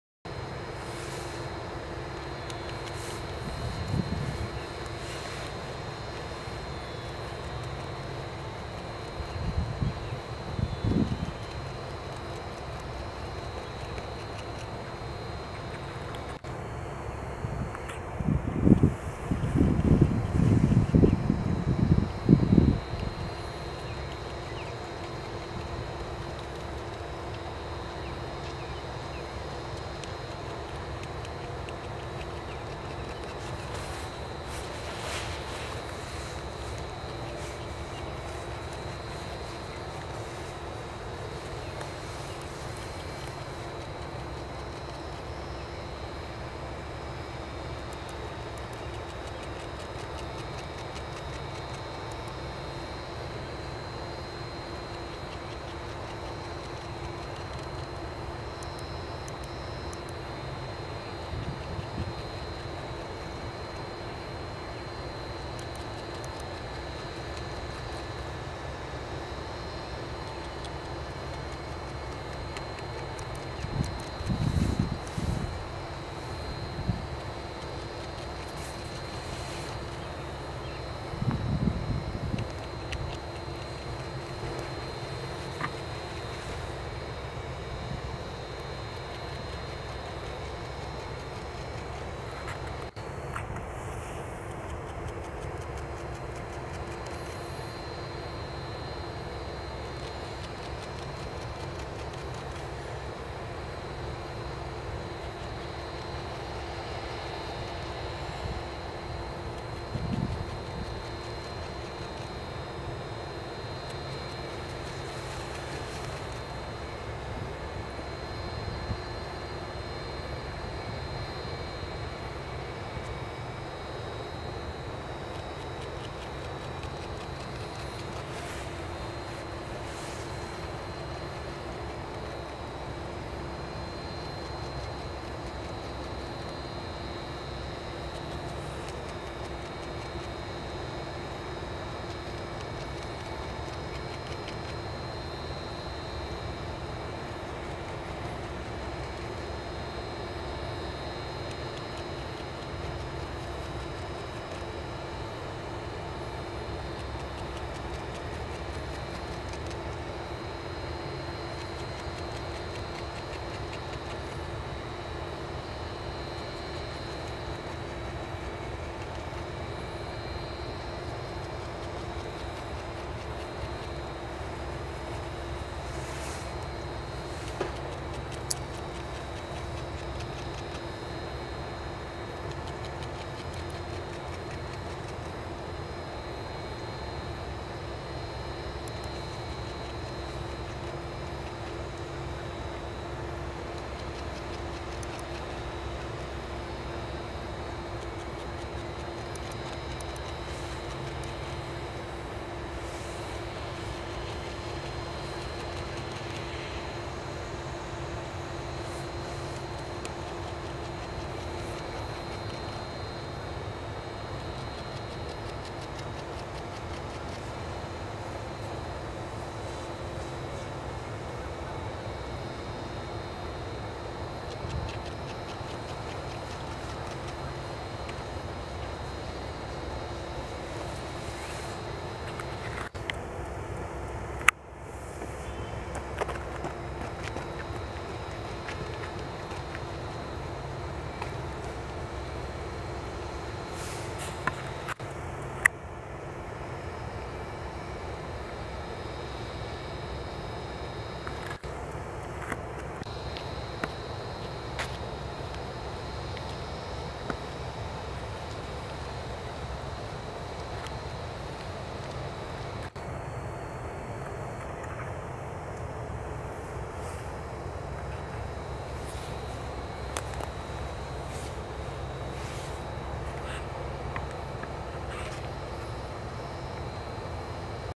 Amazing BEAVER Video: Close up on Tongue, Teeth and Hands as it debarks a branch.
This beaver was sitting in the pond eating the bark off of a branch.  It is incredible to see how neatly he is able to strip the thin layer of bark off of the branch to eat it.  Watching his tongue go in and out as he works; it is amazing he does not bite his tongue.  I also like watching his llittle hands (well, actually they are the front feet) as they handle the branch with great dexterity.